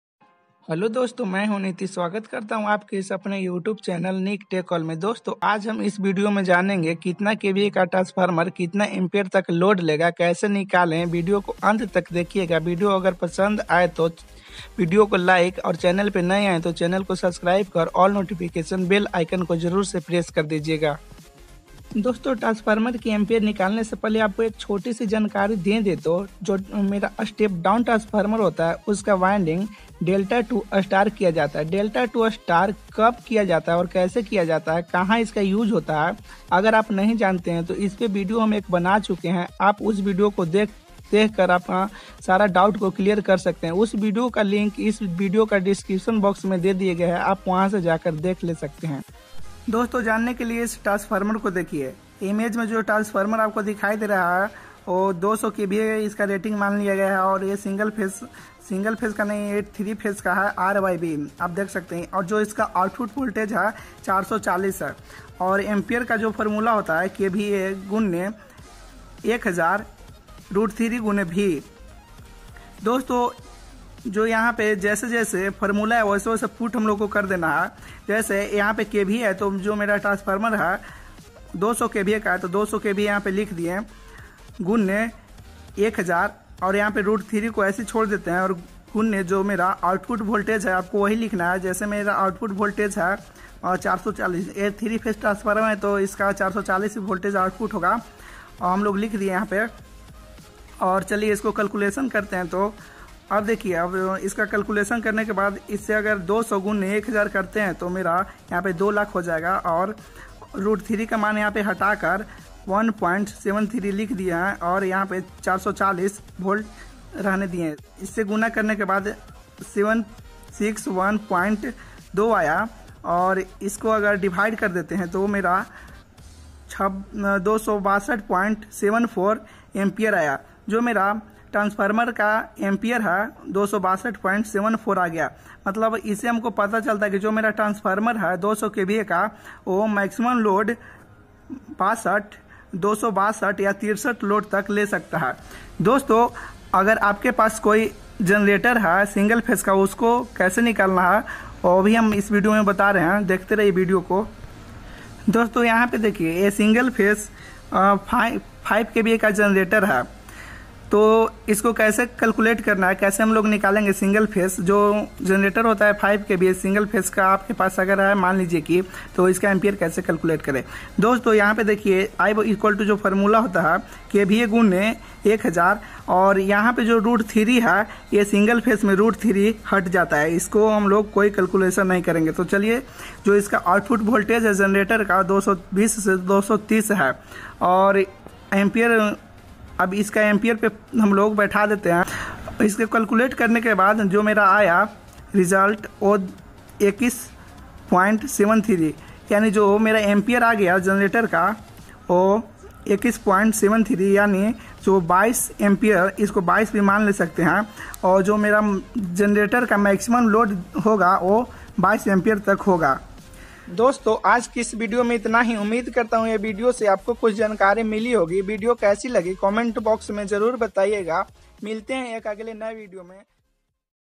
kva to ampere single phase and three phase system calculation in hindi
dosto is video me transformer ka kva se usaka current nikalna btaya gya hai ki transformer kitna current ta load le sakta hai.

star or delta kaha use hota hai video dekhne ke liye is vala link pe clik kare

 my channel niktechall